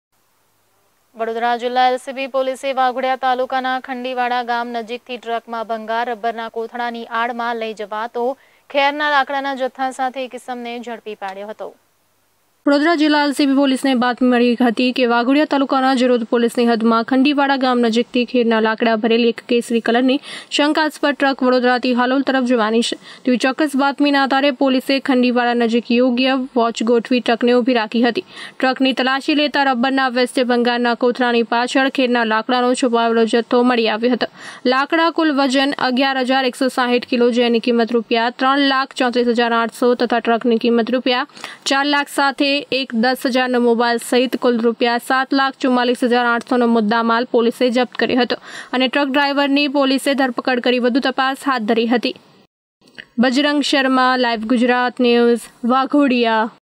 વાઘોડિયા: વડોદરા હાલોલ રોડ ઉપર એલ.સી.બીને મળી સફળતા
વડોદરા જિલ્લા એલસીબી પોલીસે વાઘોડિયા તાલુકાના ખંડીવાડા ગામ નજીકથી ટ્રકમા ભંગાર રબ્બરના કોથળાની આડમાં લઈ જવાતો ખેરના લાકડાના જથ્થા સાથે એક ઇસમને ઝડપી પાડ્યો હતો.